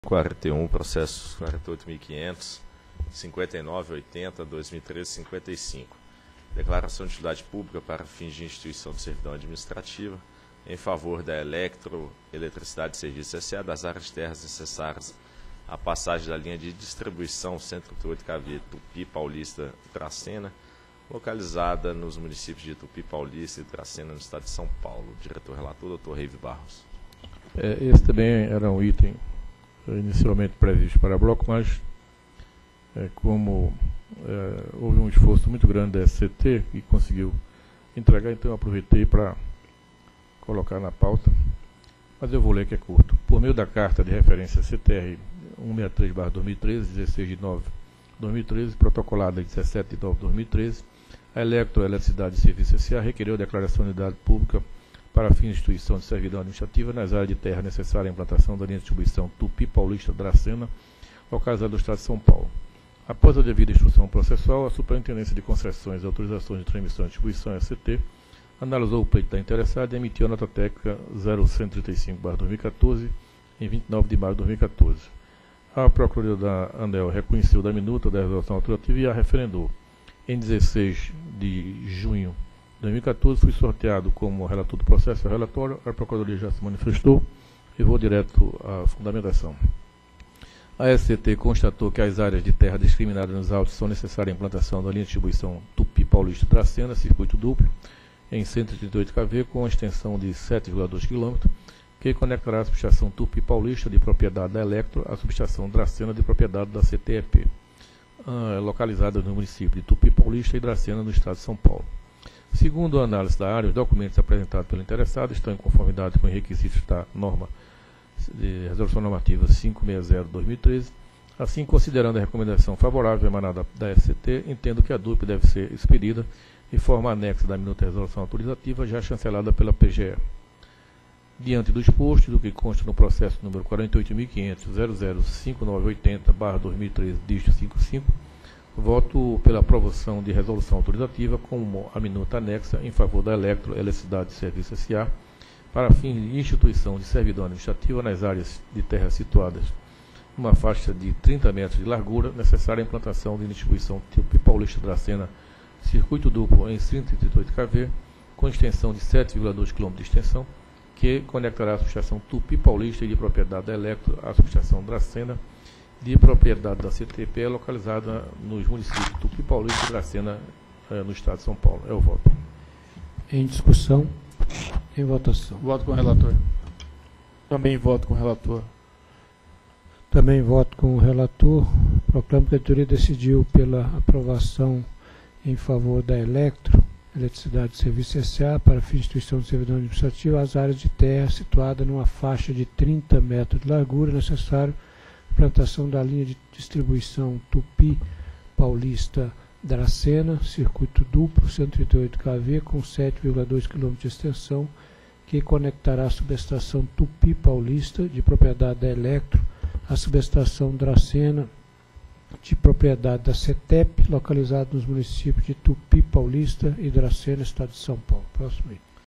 Item 41 - 22ª Reunião Pública Ordinária ANEEL 24/06/2014
Assunto: Declaração de Utilidade Pública, para fins de instituição de servidão administrativa, em favor da Elektro Eletricidade e Serviços S.A., das áreas de terra necessárias à passagem da Linha de Distribuição 138 kV Tupi Paulista -- Dracena, localizada nos municípios de Tupi Paulista e Dracena, estado de São Paulo. Relator: Diretor Reive Barros dos Santos.